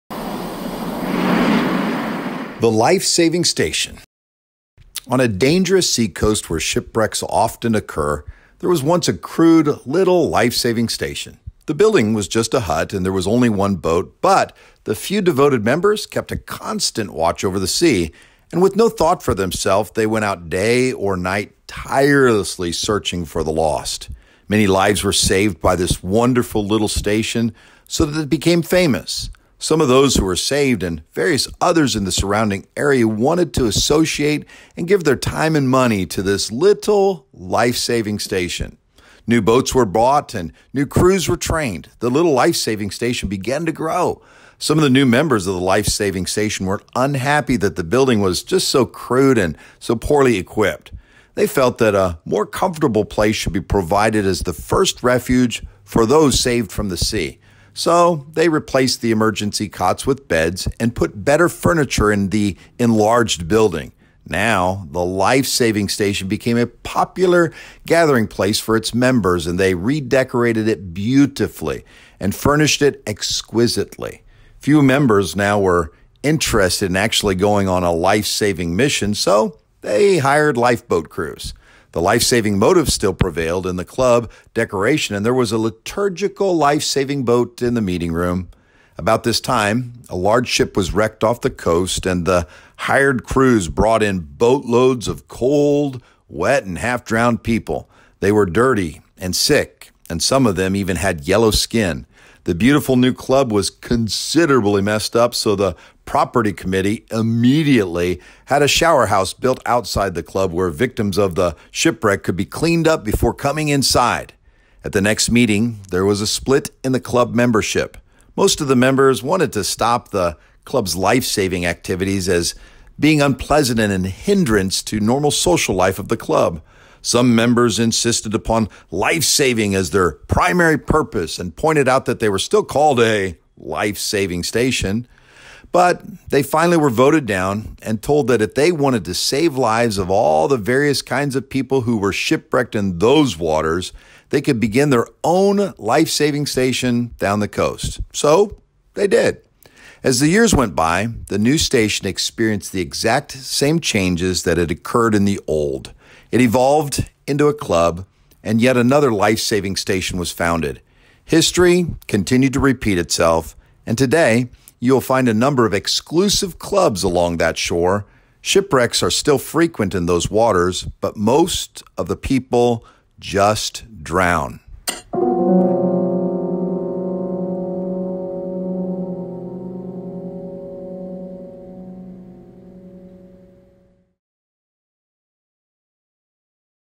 The Parable of the Life-Saving Station -The Big Picture
LIFE-SAVING STATION
 On a dangerous seacoast where shipwrecks often occur there was once a crude little lifesaving station. The building was just a hut, and there was only one boat, but the few devoted members kept a constant watch over the sea, and with no thought for themselves they went out day or night tirelessly searching for the lost. Many lives were saved by this wonderful little station, so that it became famous. Some of those who were saved, and various others in the surrounding area, wanted to associate and give their time and money. New boats were brought and new crews were trained. The little lifesaving station grew. Some of the new members of the lifesaving station were unhappy that the building was so crude and so poorly equipped. They felt that a more comfortable place should be provided as the first refuge of those saved from the sea. So they replaced the emergency cots with beds and put better furniture in an enlarged building. Now the lifesaving station became a popular gathering place for its members, and they redecorated it beautifully and furnished it exquisitely. Fewer members were now interested in going on lifesaving missions, so they hired lifeboat crews. The lifesaving motif still prevailed in the club decoration and there was a liturgical lifesaving boat in the meeting room. About this time, a large ship was wrecked off the coast, and the hired crews brought in boatloads of cold, wet and half-drowned people. They were dirty and sick and some of them had yellow skin. The beautiful new club was considerably messed up. So the property committee immediately had a shower house built outside the club where victims of shipwreck could be cleaned up before coming inside.
 At the next meeting, there was a split in the club membership. Most of the members wanted to stop the club's lifesaving activities as being unpleasant and a hindrance to the normal social life of the club. Some members insisted upon lifesaving as their primary purpose and pointed out that they were still called a lifesaving station. But they were finally voted down and told that if they wanted to save lives of all the various kinds of people who were shipwrecked in those waters, they could begin their own lifesaving station down the coast. They did. As the years went by, the new station experienced the same changes that had occurred in the old. It evolved into a club, and yet another lifesaving station was founded. History continued to repeat itself, and today you will find a number of exclusive clubs along that shore. Shipwrecks are still frequent in those waters, but most of the people drown!
Theodore Wedel, Warden, College of Preachers
 Washington Cathedral, Washington, D.C.

Here is a link to make a donation to Grace Baptist Temple

Pastor Matt Nettesheim
Address: 1101 Burns Street, Chesapeake, VA 23320